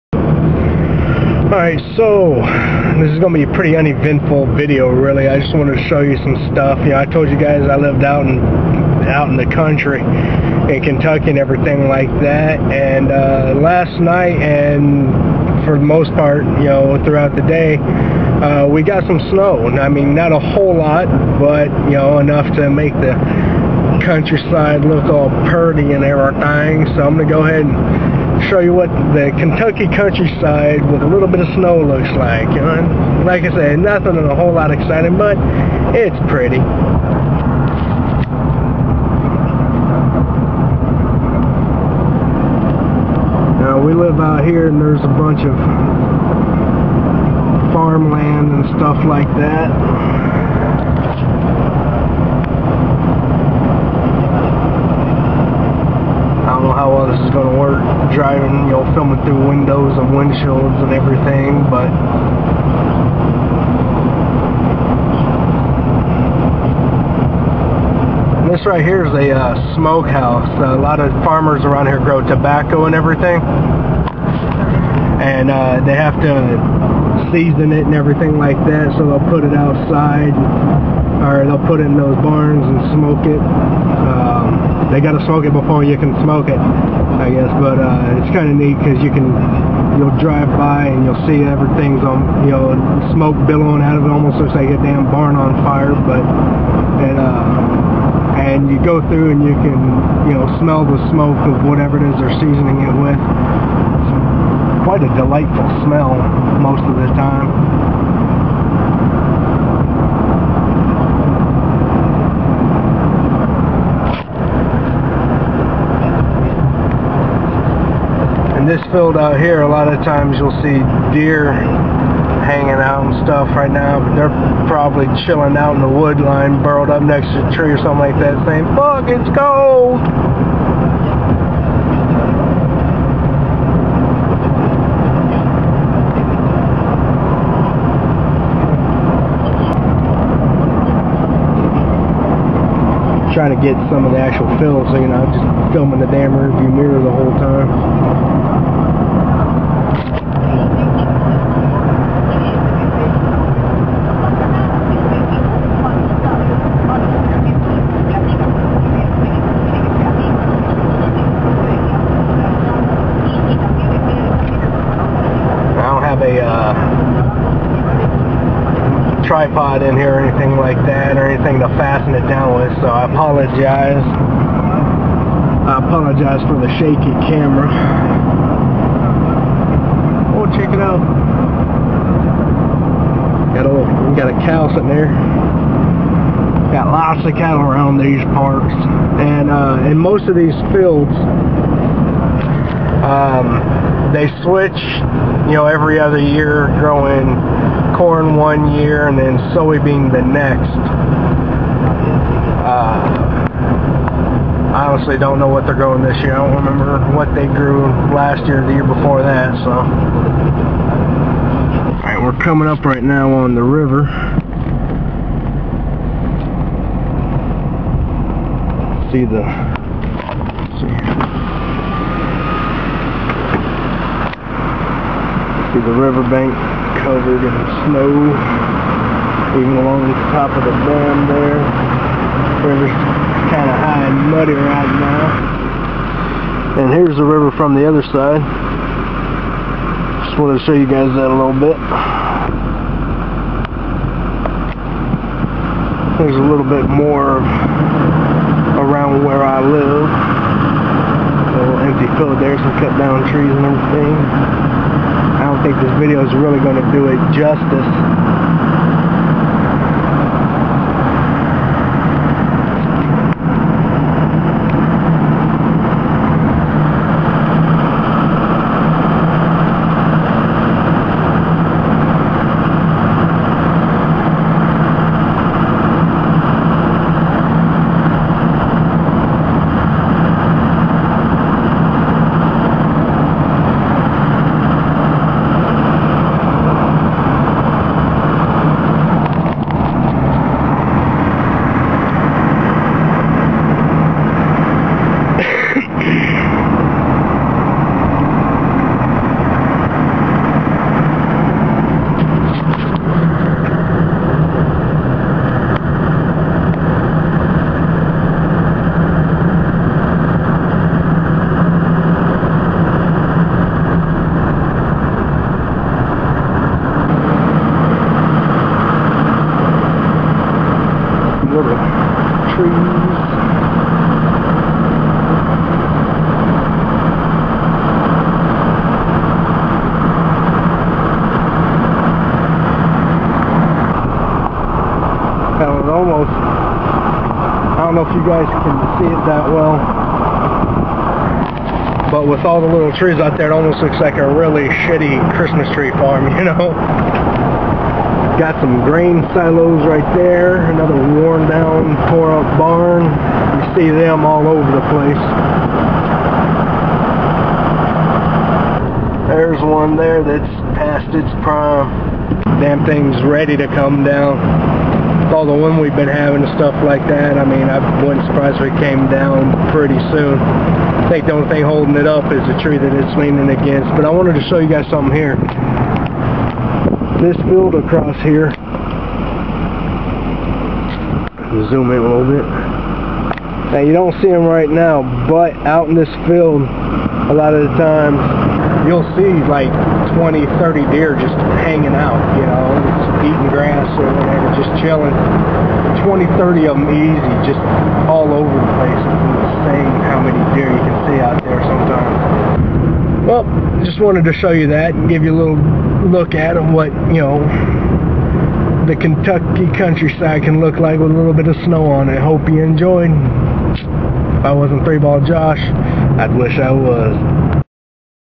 Snow Day...Sorta
Just showing the KY countryside with a little bit of snow on the ground.